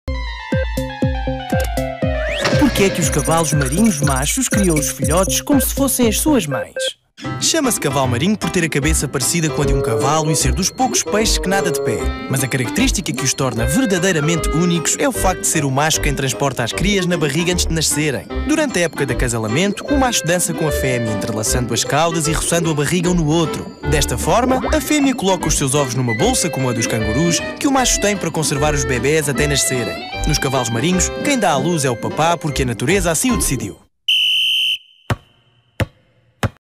Porque é que os cavalos-marinhos machos criam os filhotes como se fossem as suas mães?
Nos cavalos-marinhos, quem dá à luz é o papá, porque a Natureza assim o decidiu.